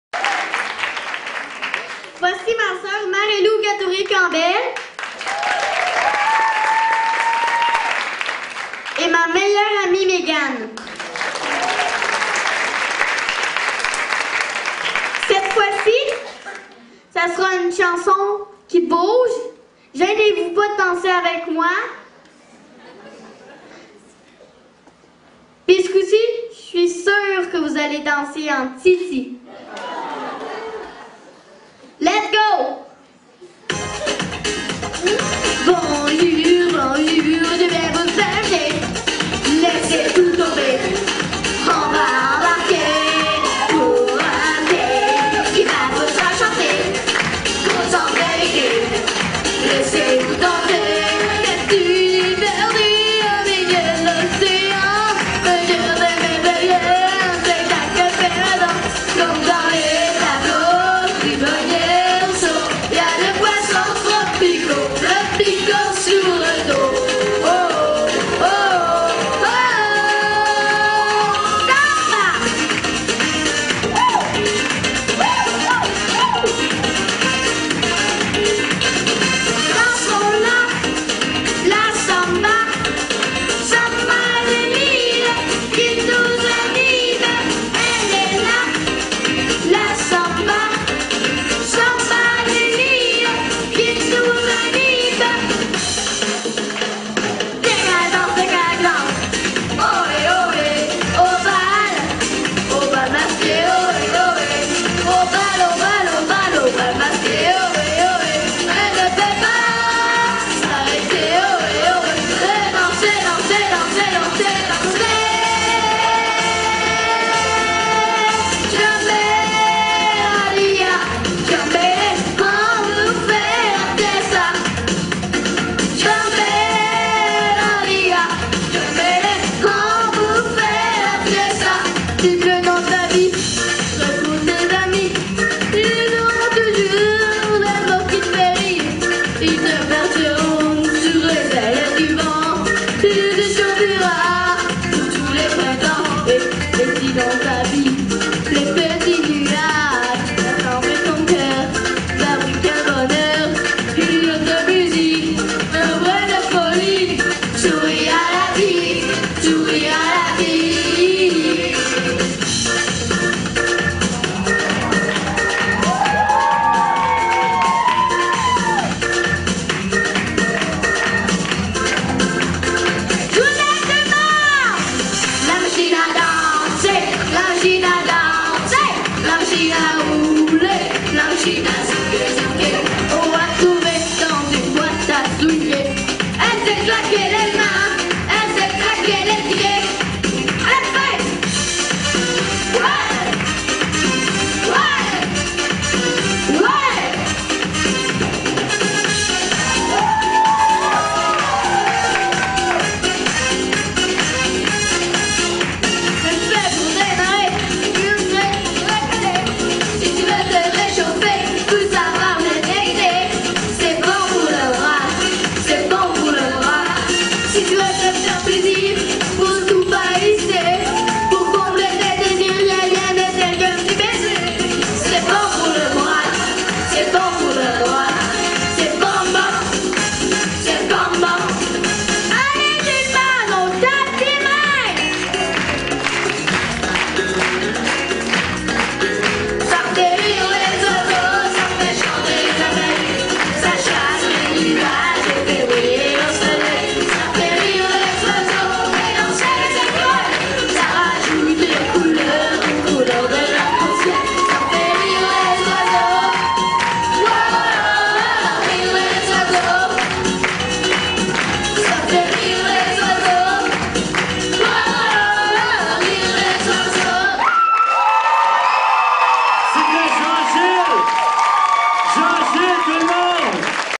Société Canadienne du Cancer mars 2012: Megamix de la Compagnie Creole - Jean Gilles Gadoury 6 ans
Megamix de la Compagnie Creole - Jean Gilles Gadoury 6 ans